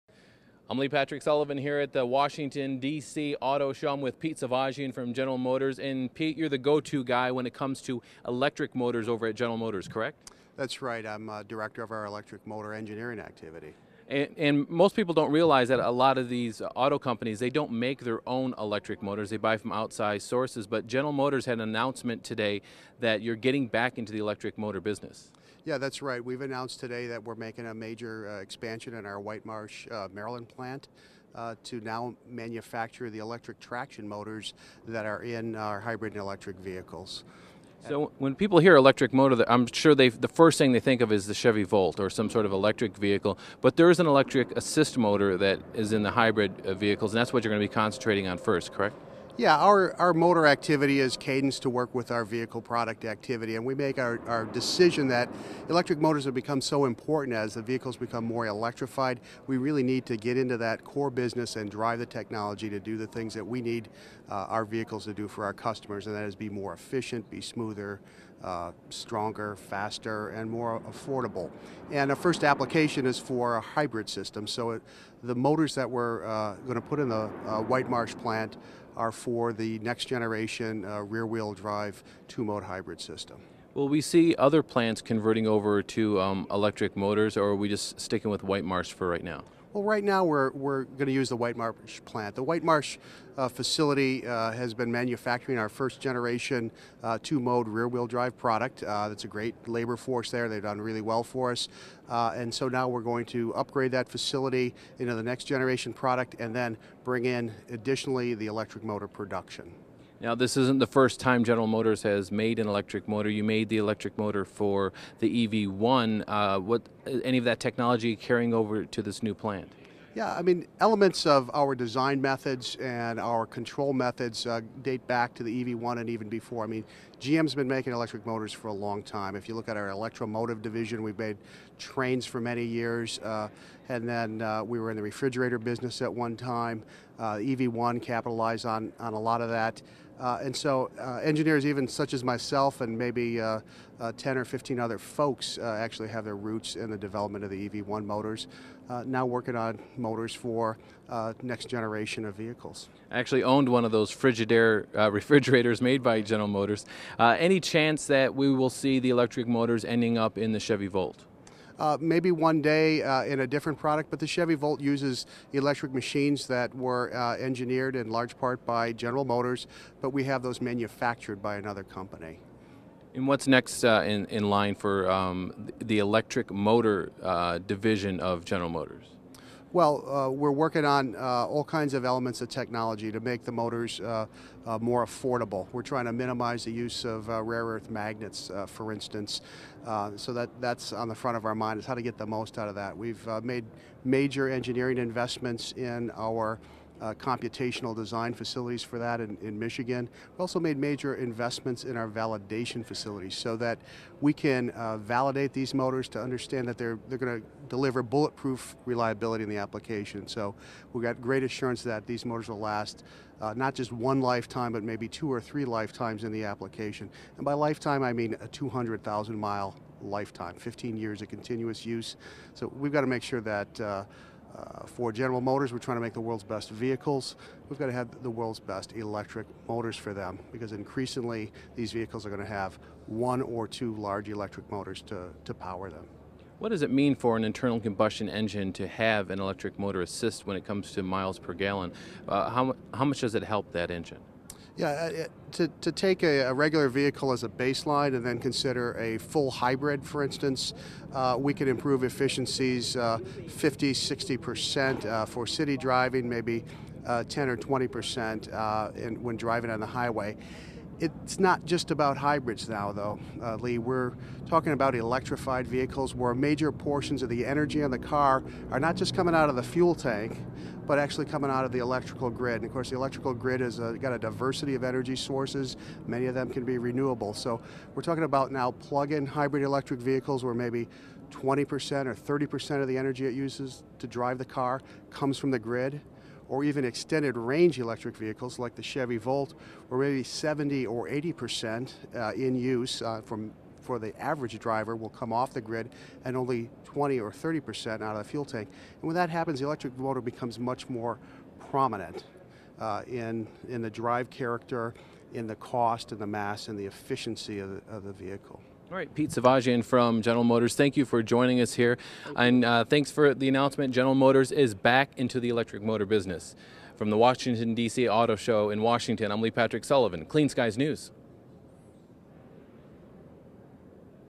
GM's Pete Savagian on Electric Motors
Pete Savagian from General Motors discusses GM's plans to get back in the electric motor business.  The auto giant announced plans to turn the White Marsh Transmission Plant into the new home of their electric motor division.  The motors will be used for GM's new two-mode hybrid vehicles as well as other applications.